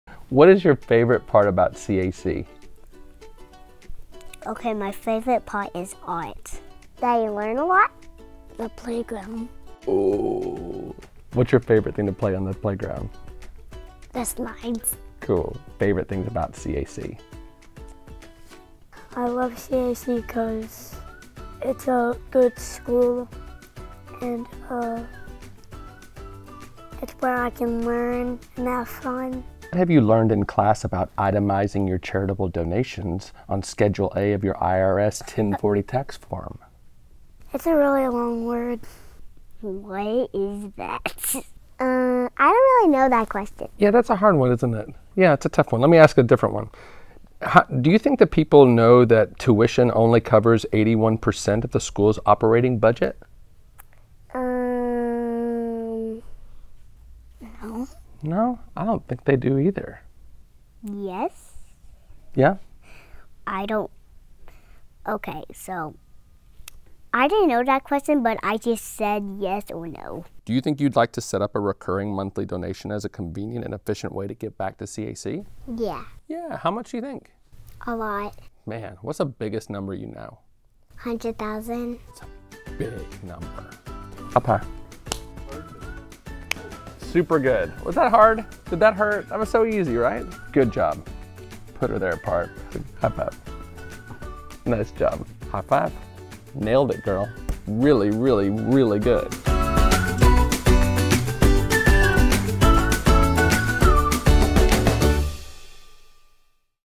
CAC Giving Day 2024
We sat down with some CAC elementary students to ask them important questions, such as: What do you love about school? What's your favorite snack? Do you know how to mark itemized charitable donations on your 1040 tax form?

Thank you for helping us meet our goals this Giving Day. or give us a call. Go Mustangs!